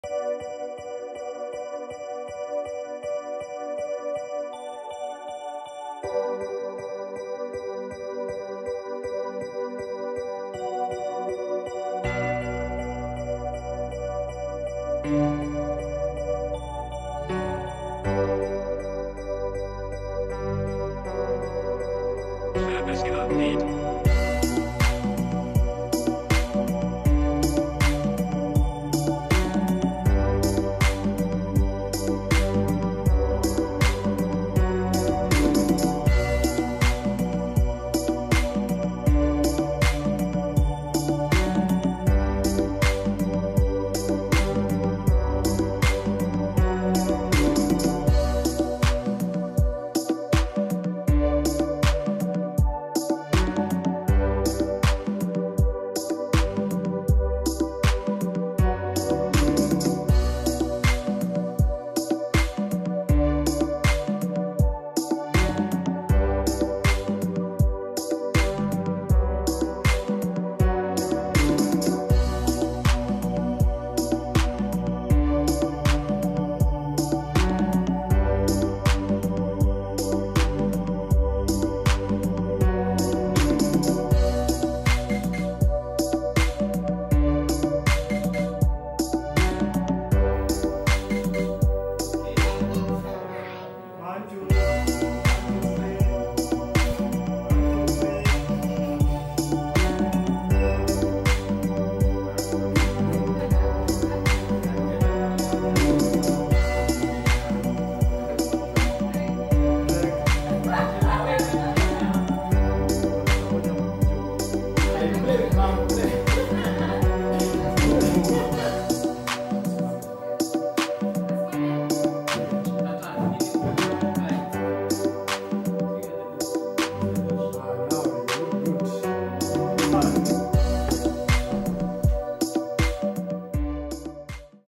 Studio Family Portraits Photoshoot Session :: Make Up By Jacque kanana at Mellow Heights Ngara
Recent behind the scene of the family portrait photoshoot photography in the studio at the mellow heights Ngara road Nairobi Kenya, makeup was done by Jacque Kanana of the Make-Up By Kanana